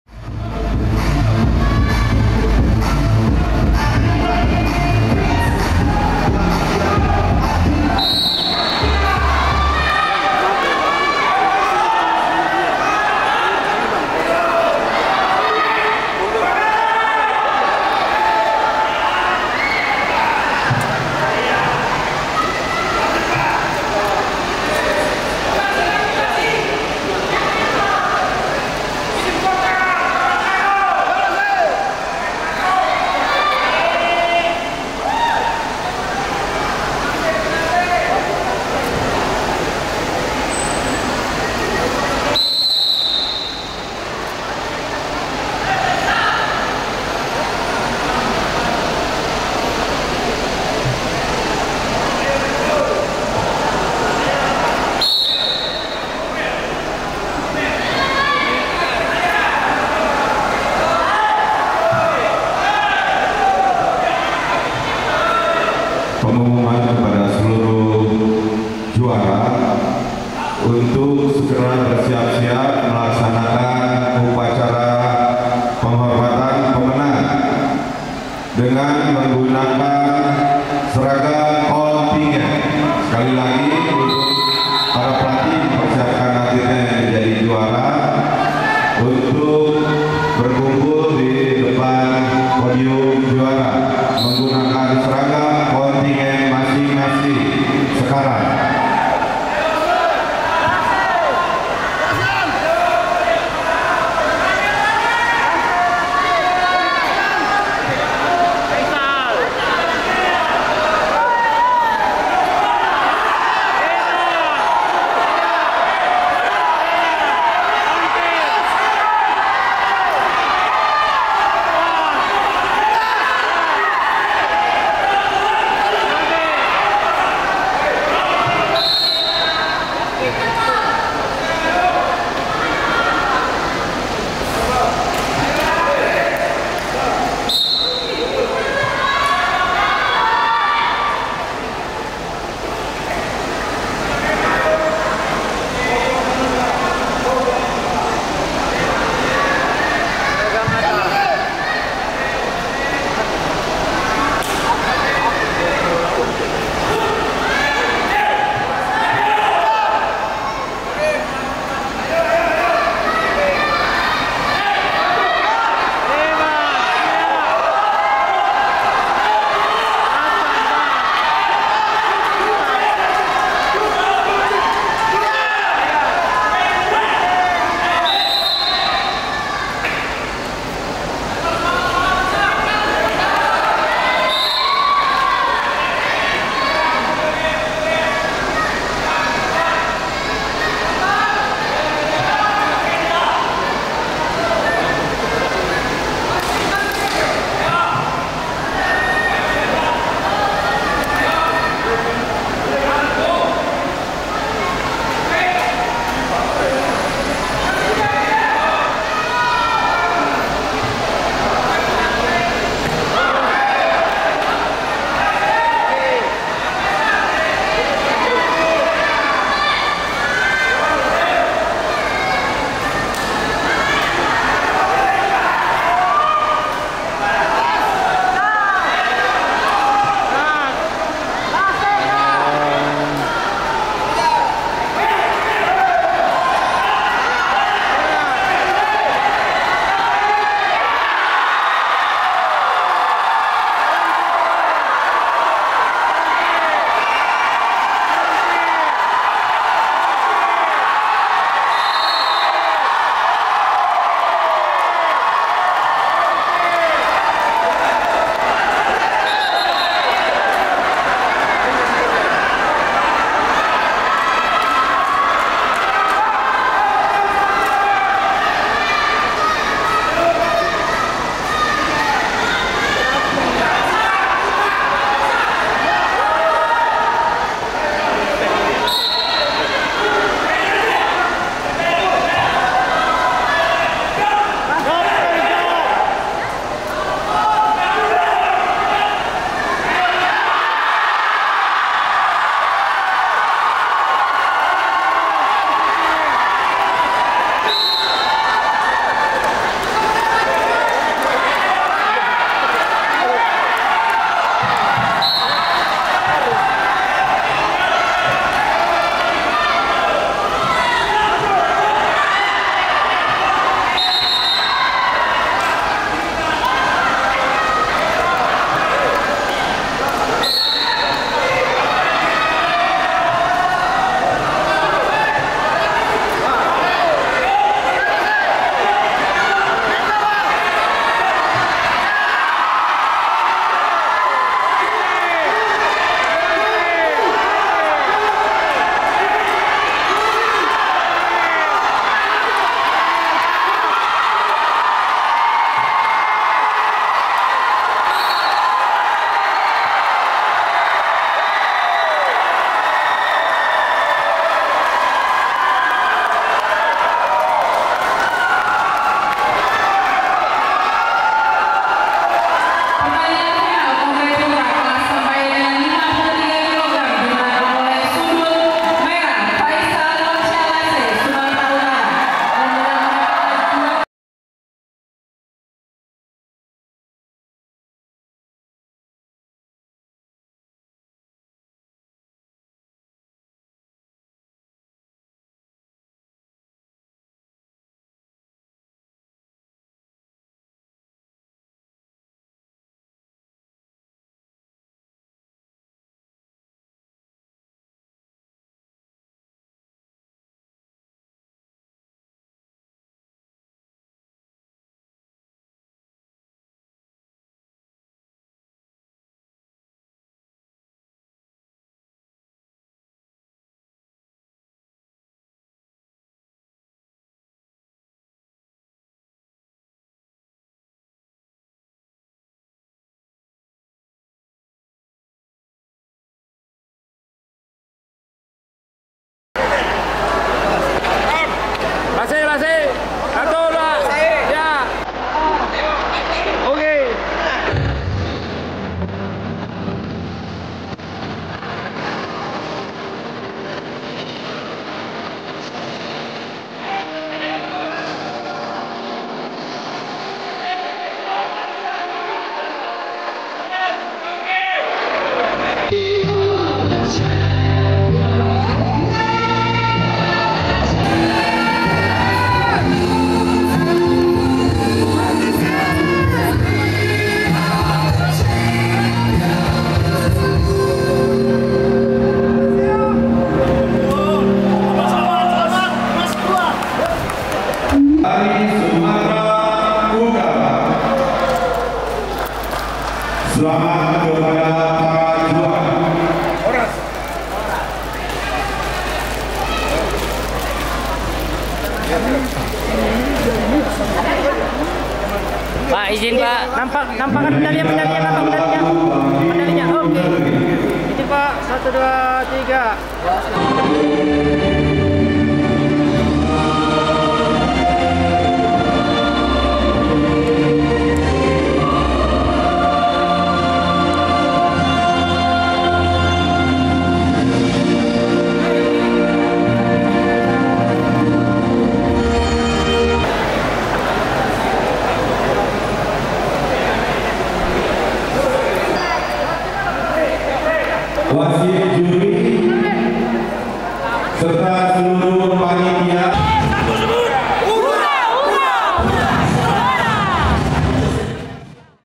FINAL CABOR SAMBO COMBAT PUTRA 53 kg - SUMATERA UTARA VS SULAWESI UTARA | PON 2024
Pekan Olahraga Nasional (PON) XXI Aceh-Sumut 2024 resmi mempertandingkan cabang olahraga (cabor) Sambo untuk pertama kalinya. secara resmi membuka pertandingan Sambo di Gedung Olahraga Manunggal Langkat Berseri, Stabat, pada Senin (16 September 2024).

Combat Putra (53 Kg):

Juara

1. Faizal Iwansyah Lase (Provinsi Sumatera Utara)
2. N. Yeremia Kaunang (Provinsi Sulawesi Utara)
3. Bangkit Dewangga (Provinsi Jawa Timur)
4. Yudistira (Provinsi Papua Selatan)